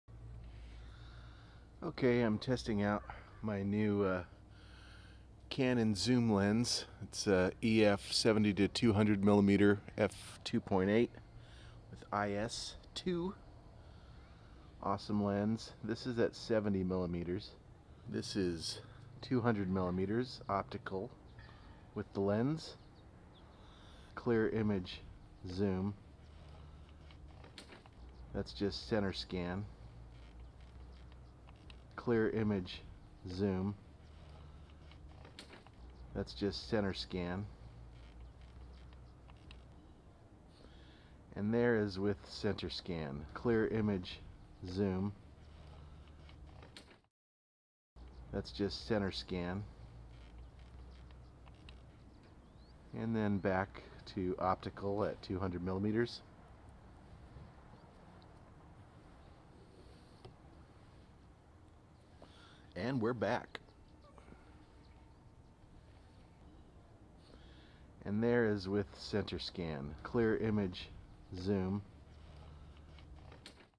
Sony FS5 Center Scan vs Clear Image Zoom Test 1
Here’s a quick test with the Sony FS5 camera and the Canon 70-200mm f/2.8 IS II lens. I wanted to see how far it could reach with the FS5’s 2 extra zoom features. When in 1080p mode you can use something called “Center Scan” where it just records a 1920x1080 portion of the sensor, which is like adding a 200% zoom. Then there is "Clear Image Zoom", which lets you use the zoom rocker to zoom in 200% also, but that’s more like a digital zoom and doesn’t look as good as Center scan. But you can also use them both at the same time for a big zoom like a 600mm lens. Combining center scan and clear image zoom doesn't look very good though.